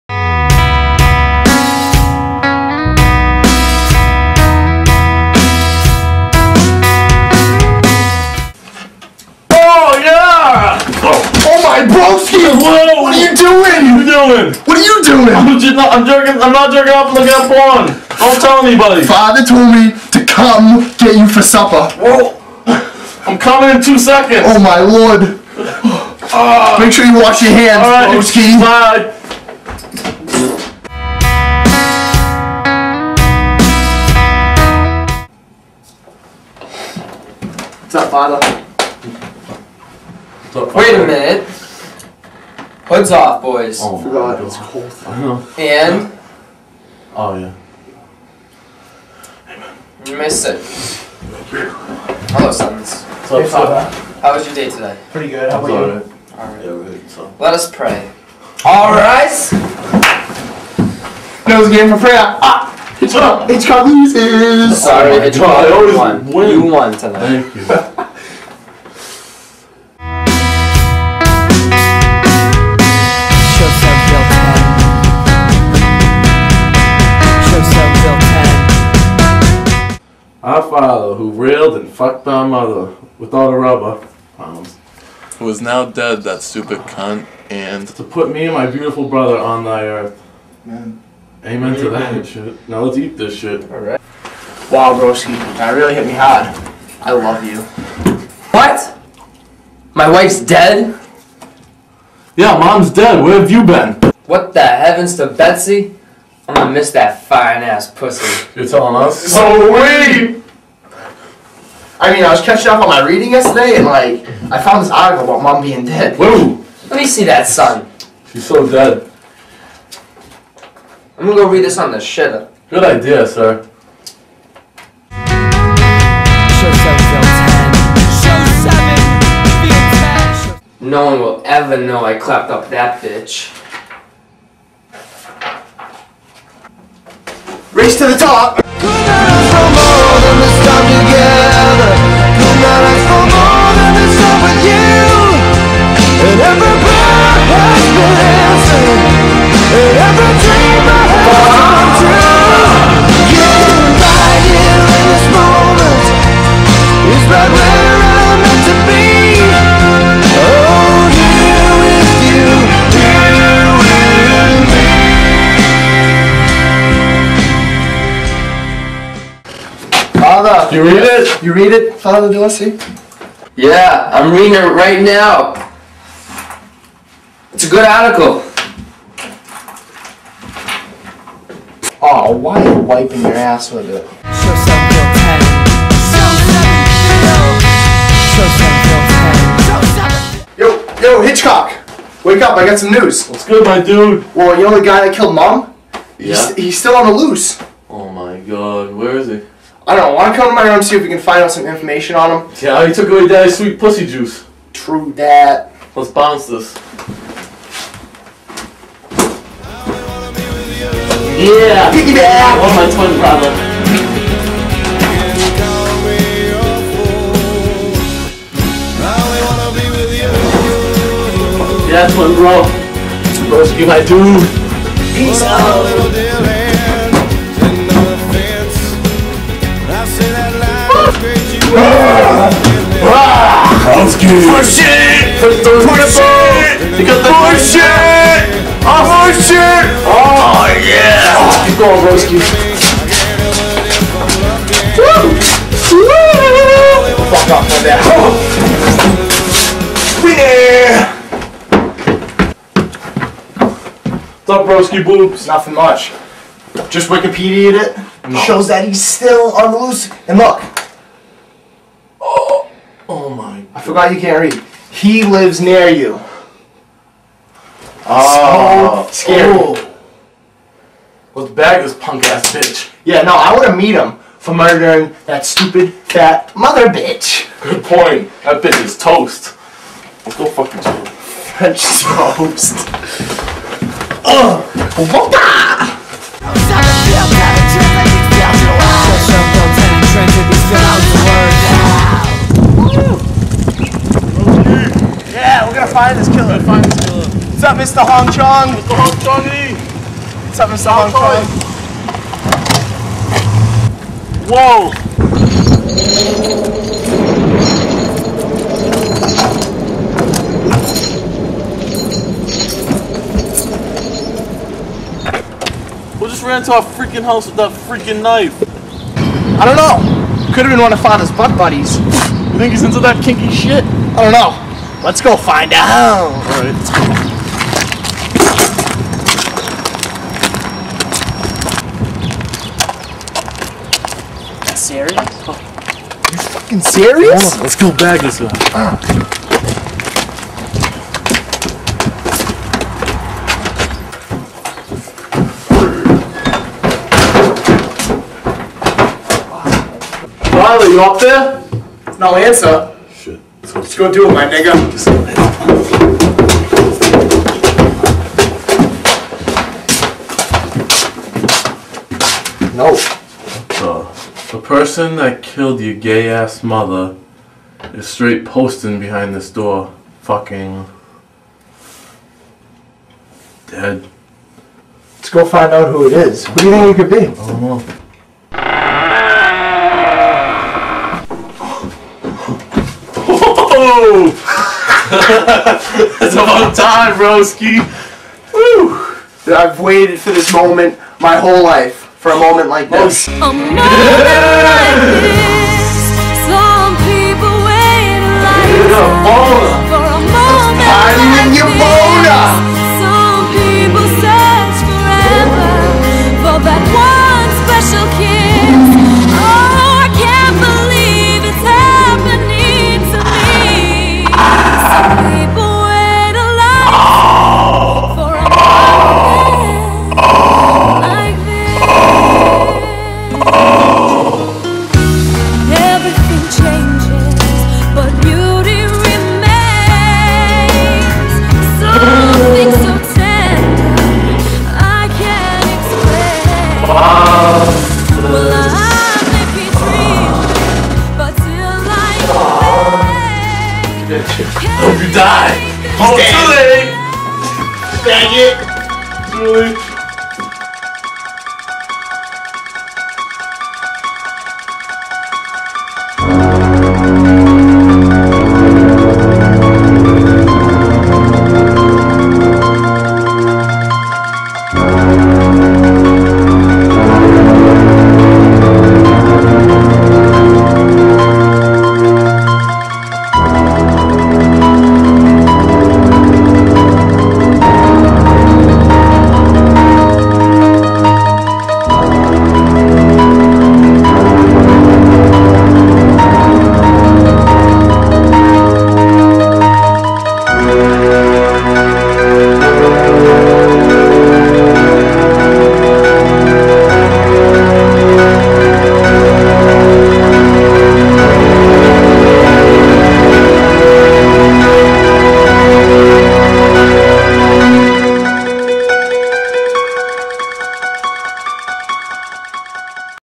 Parents
Two sons who love there dad, try to find out who killed there mother.